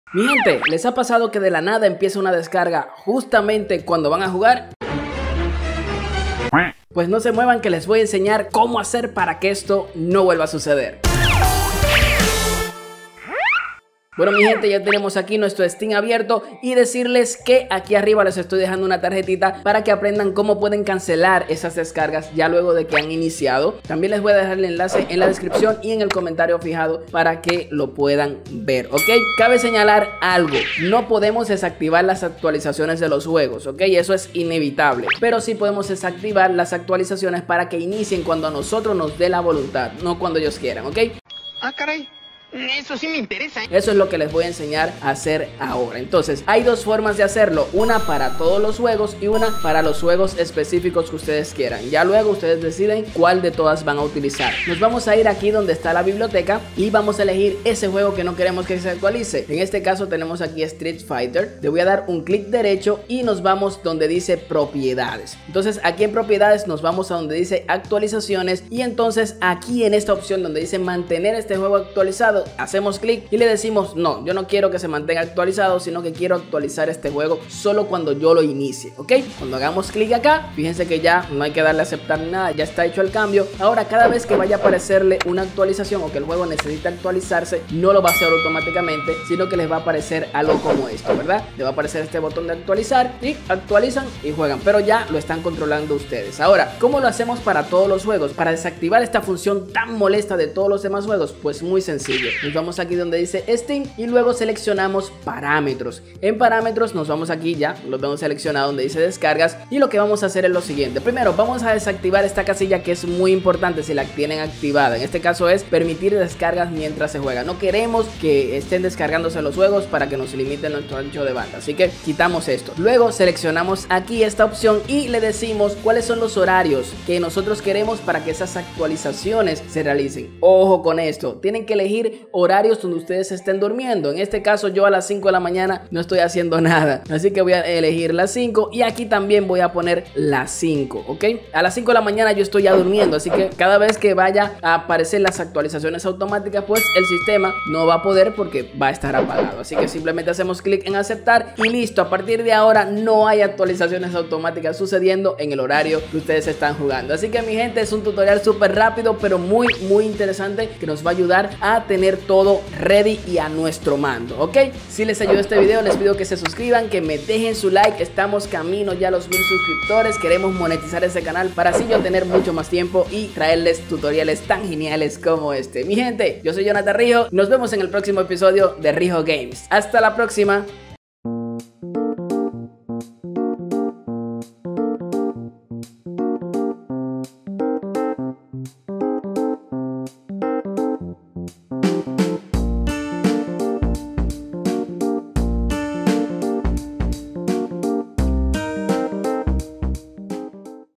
Como DESACTIVAR (Actualizaciones Automáticas) de JUEGOS en STEAM ❌ BLOQUEAR Actualización
Aprende de forma sencilla Como DESACTIVAR la (Actualizaciones Automáticas) de Juegos en Steam. Este es un tutorial muy corto y rápido, pero seguro de mucha utilidad.

Abecés lo único que deseamos es jugar nuestro videojuego favorito, y justamente en ese momento se le ocurre aparecer esas actualizaciones, pero con este método las podrán bloquear para que YA NO HAYA MAS ACTUALIZACIONES AUTOMATICAS.